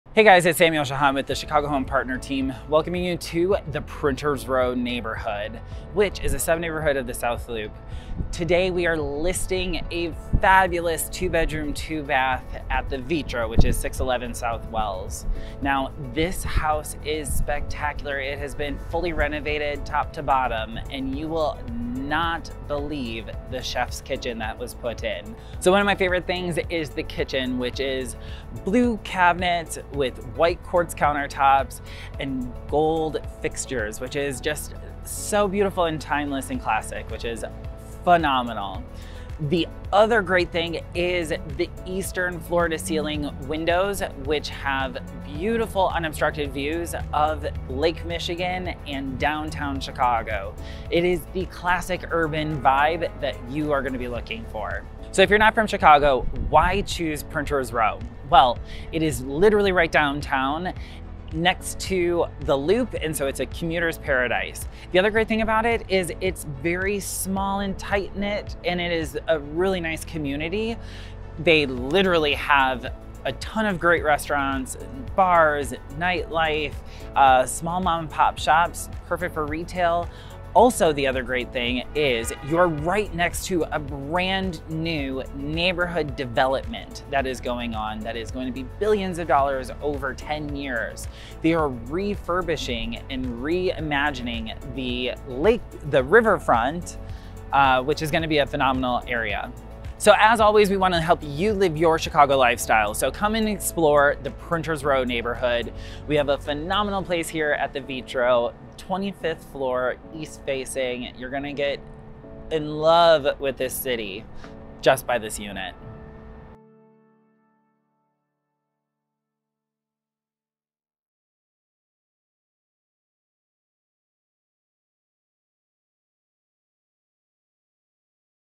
Chicago Lifestyle: Printers Row / South Loop Condo for Sale - 611 S Wells St Unit 2509
Chicago Lifestyle is so happy to present The Vetro, 611 S Wells St Unit 2509 for sale!

2 Bed / 2 Bath
$445,000 + $30,000 for garage parking.

Info:

Welcome to the Vetro in Chicago's Printers Row neighborhood! Unit 2509 stands out amongst the other units in the building, because the previous owner reconfigured the entire kitchen into the quintessential chefs kitchen. Are you ready to prepare your favorite meal with loved ones? Let's talk about the kitchen. The finished package is drop dead stunning - rooted with dark gray herringbone floors; dressed up in tuxedo styled white and cerulean blue cabinets; lined with white veined quartz countertops; and elevated with gold fixtures. A high end stainless steel appliance package and a built in pantry is the creme de La creme that makes a chef's kiss linger. Upon entry, the new wide plank, espresso hardwood floors in combination with the concrete loft lead you to stunning floor to ceiling windows that have unobstructed eastern views of the urban Chicago landscape and dazzling Lake Michigan views. The living room is open and airy with a spider, pendant chandelier ultimately being the divide between the split floorplan. To the north, the primary suite is large enough for a king size bed with ample room for additional furniture, and it features a walk in closet outfitted with custom organization systems. The en-suite bathroom has dual vanity and a deep bathtub perfect for long soaks after a long day. A second split bedroom is the perfect guest room, office or additional space! It has a large walk in closet with organizational systems as well. Outdoor space is ample with two large balconies off the living room and the primary bedroom! Never discussed, but always appreciated is that this unit has no neighboring walls which make it super private and quiet! Brand new washer / dryer in unit. Additional storage is included. Garage parking is sold separately! The Vetro is a newer construction full amenity building - 24 hour doorman, outdoor sky deck, party room, and fitness center. We can't wait for this to be your home!